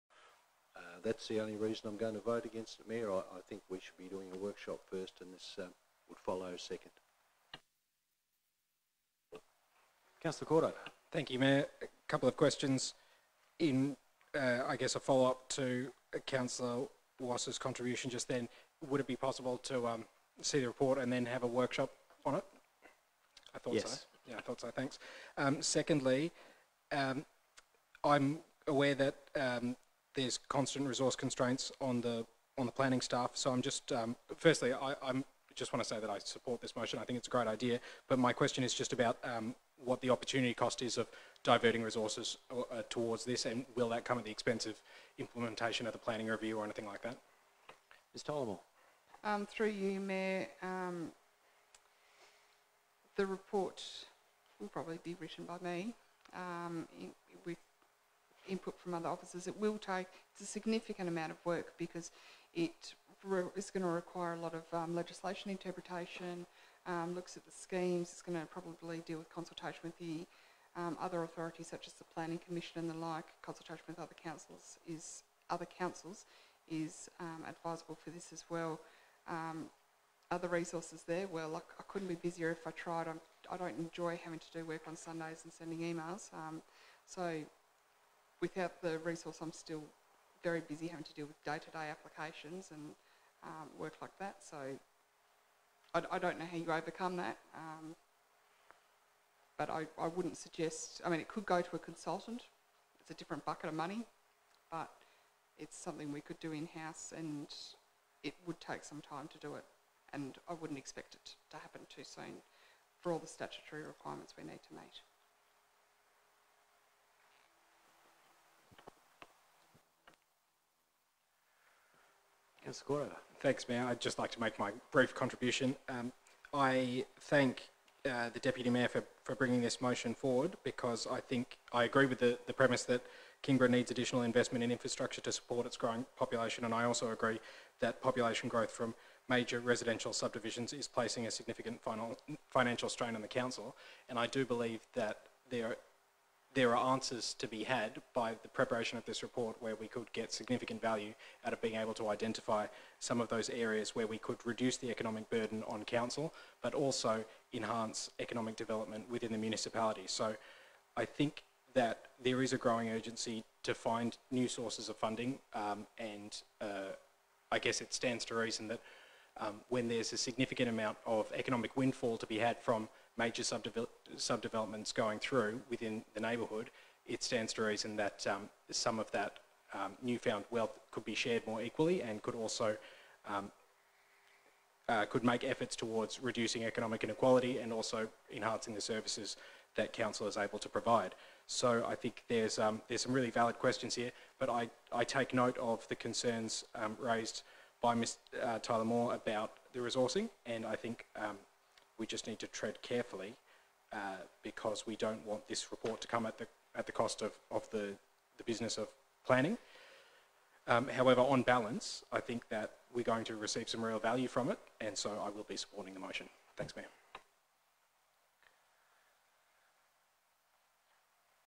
Gideon says large subdivision developers should help contribute to community infrastructure costs.
27 July 2020, Kingborough Council public meeting. Cr Gideon Cordover speaks about subdivision costs.

14 NOTICES OF MOTION
C429/14-2020
(commences at ± 1 hour, 15 minutes of audio recording)
14.1 FUNDING FOR PUBLIC INFRASTRUCTURE REQUIRED TO SUPPORT LARGE SUB-DIVISIONS
Moved: Cr Jo Westwood
Seconded: Cr Amanda Midgley
That a report be provided to Council regarding options to assist in funding the provision of public infrastructure to support large new subdivisions and rezones.
The report should include, but not be limited to:
• analysis of the infrastructure requirements created by recent large residential subdivisions;
• financial analysis, including estimated revenue versus costs and time taken to achieve a return on investment to Council from recent major residential subdivisions;
• potential policy solutions to address funding of public infrastructure necessitated by large subdivisions;
• options currently available within existing legislation to implement changes to the way Council funds and delivers infrastructure, including land value capture options, infrastructure impact fees and public private partnerships; and
• what legislative or other changes are required to enable Council to undertake value capture arrangements from large residential subdivisions by re-allocating at least part of the burden of funding for public infrastructure requirements.
In Favour: Crs Dean Winter, Jo Westwood, Sue Bastone, Gideon Cordover, Flora Fox, Amanda Midgley, Christian Street and Paula Wriedt
Against: Cr Steve Wass
CARRIED 8/1